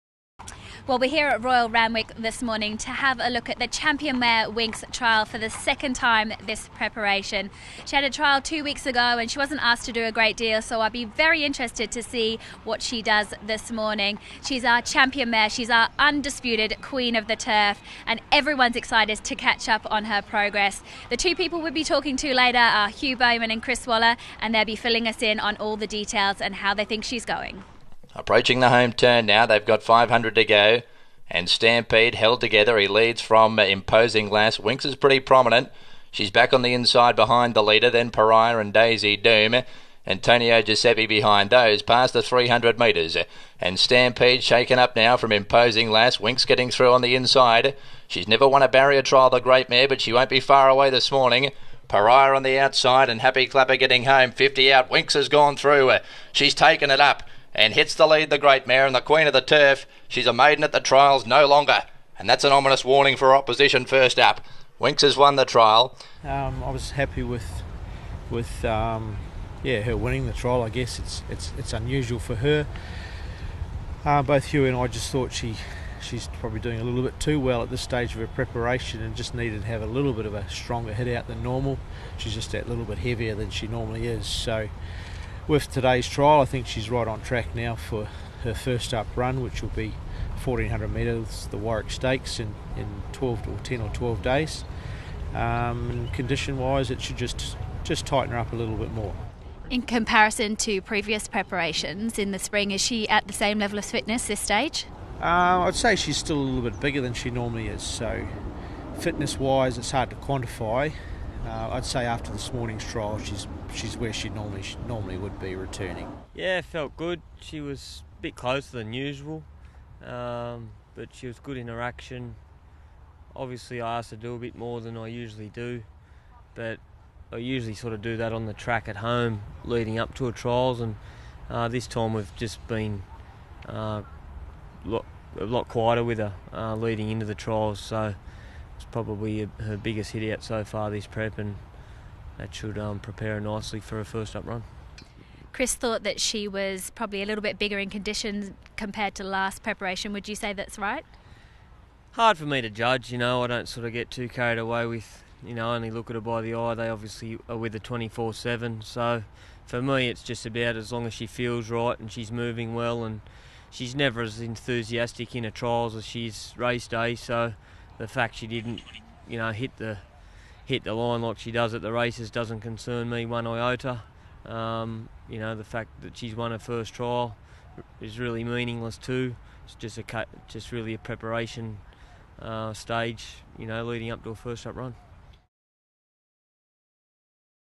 Winx Barrier Feature for August 7, 2017
Listen to connections of Winx discuss her recent Barrier win and her form as she prepares to return to the races!
Video is courtesy of Sky Racing